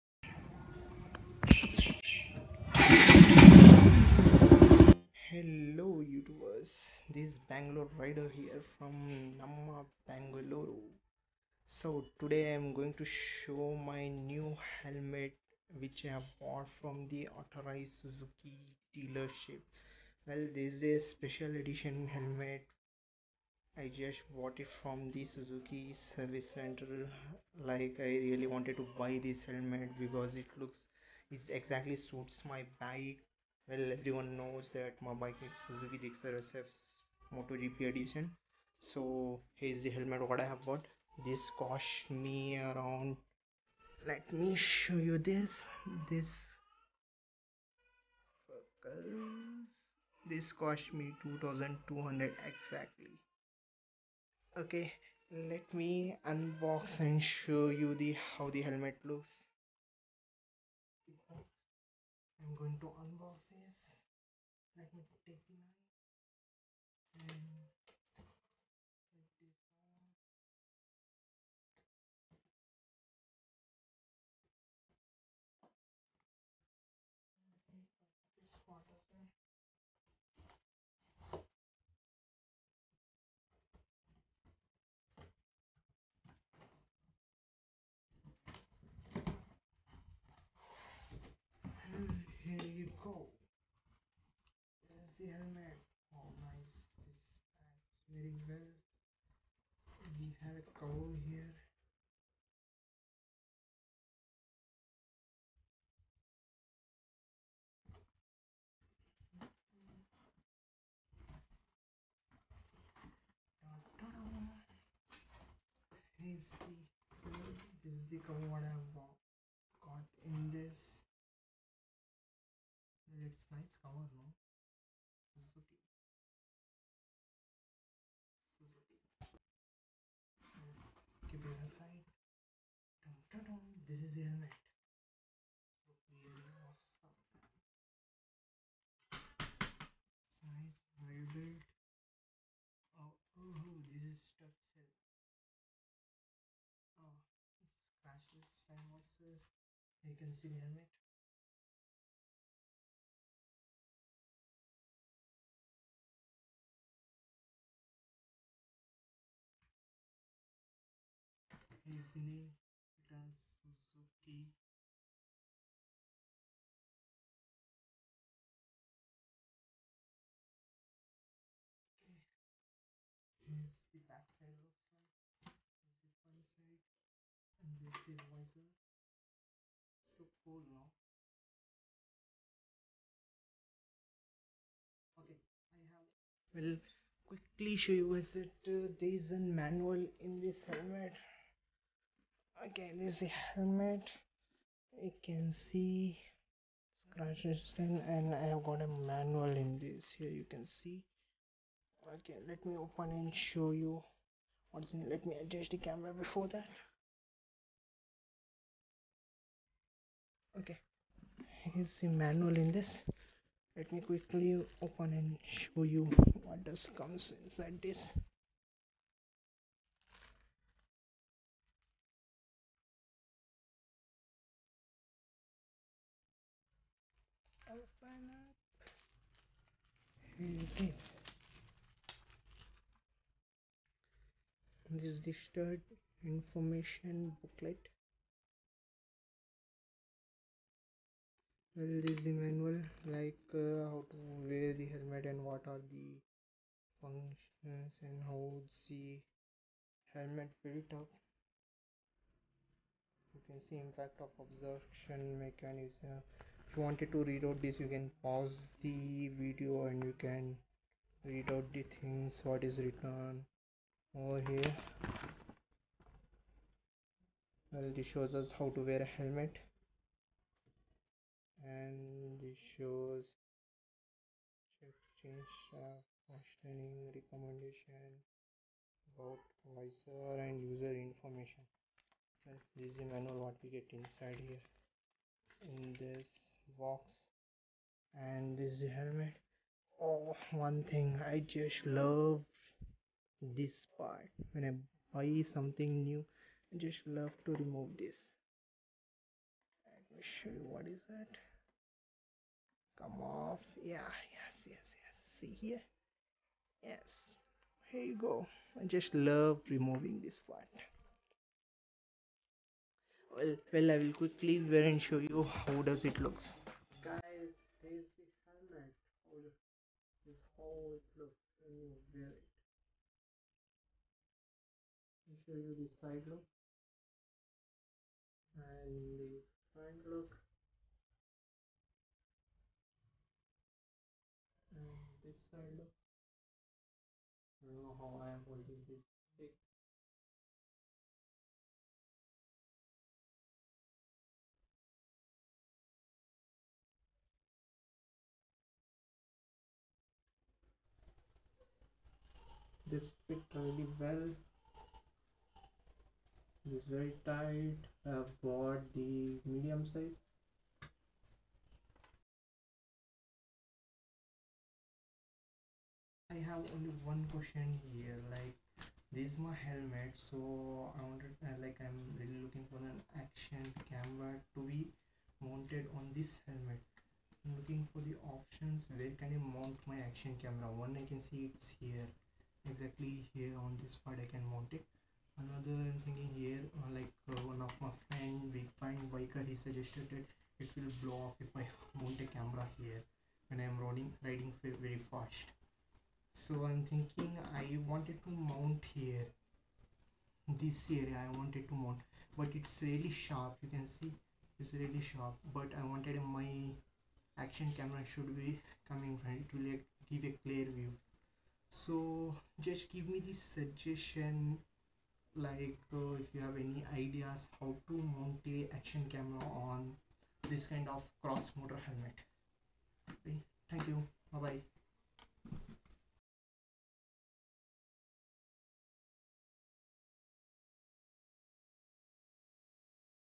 Suzuki gixxer SF helmet | Full review | Moto GP special edition | Unboxing.
Bought this helmet from KH Road Suzuki Showroom (Bangalore)  for Rs2199, please do comment below if you have any questions regarding this helmet and if you like the video please hit the like button share the video and subscribe to my channel.